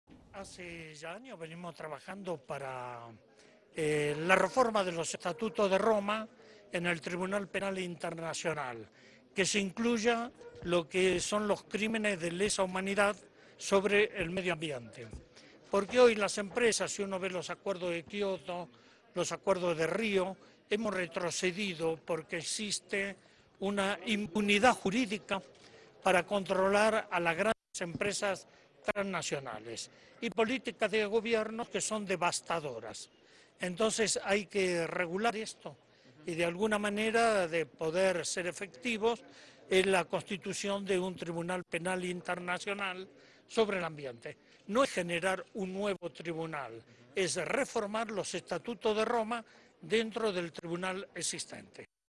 Pérez Esquivel plantea reformar el estatuto de Roma - Lunes 12 octubre 2015
(Abya Yala Digital).- El  premio Nobel de la Paz, Adolfo Pérez Esquivel, planteó el lunes reformar el Estatuto de Roma de 1998, para incluir los delitos medio ambientales como crímenes de lesa humanidad.

A su juicio, las sanciones contra la depredación del medio ambiente y sus efectos catastróficos sobre la humanidad no necesariamente requiere la creación de un nuevo Tribunal de Justicia Climática, como proponen los delegados que asisten a la II Conferencia Mundial de loa Pueblos sobre el Cambio Climático y la Defensa a la Vida, que se realiza en Tiquipaya.

La Corte Penal Internacional es la primera Corte permanente, independiente, con la capacidad de investigar y llevar ante la justicia a quienes cometan las violaciones más graves en contra del derecho internacional humanitario, a saber crímenes de guerra, crímenes de lesa humanidad y genocidio.

Bolivia convocó a autoridades, científicos, académicos, juristas, organizaciones sociales y a la sociedad civil a participar en ese foro previo a la Cumbre Climática de la ONU a realizarse en diciembre en París. (Video)